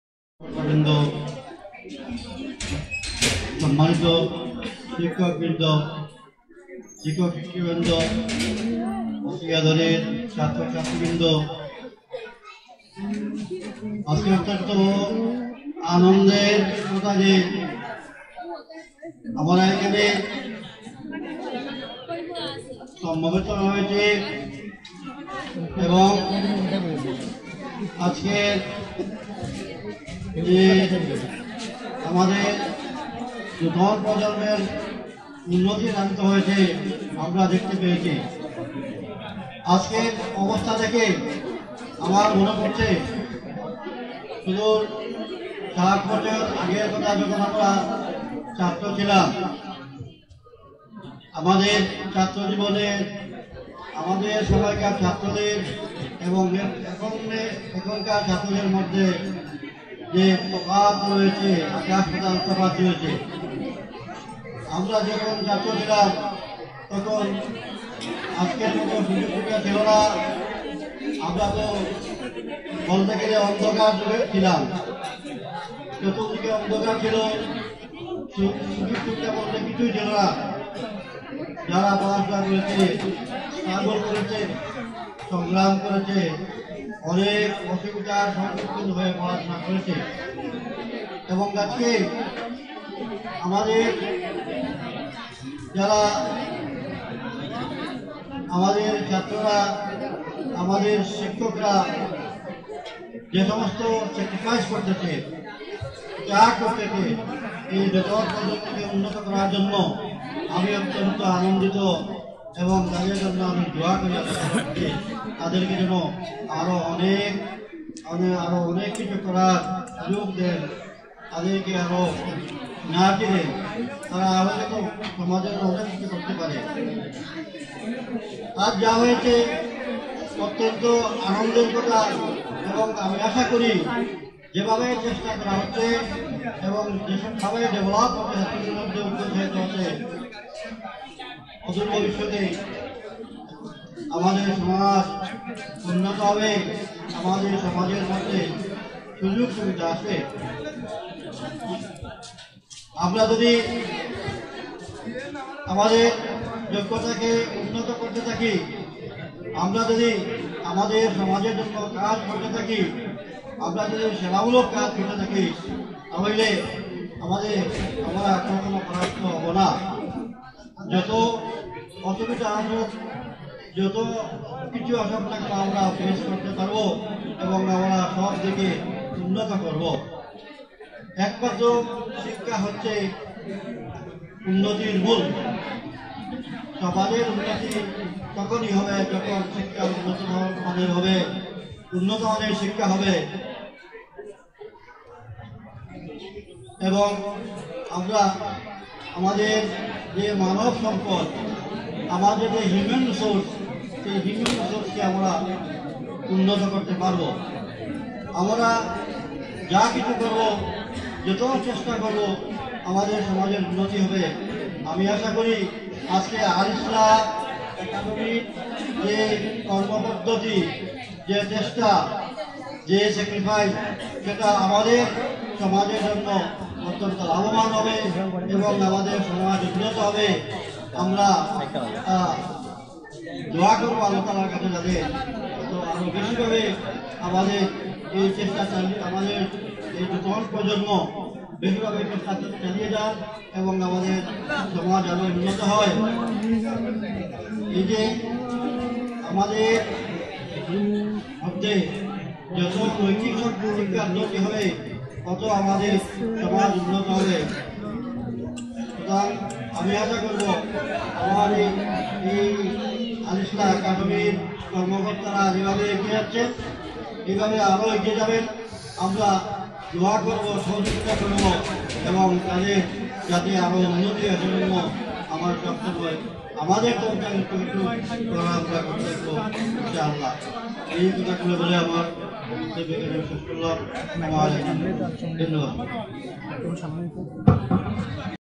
Speech of Dr. Maulana Matiur Rahman Former Principal Karimganj College || Al-Islah National Academy
GOOD NEWS !!!

In this video we are going to share the .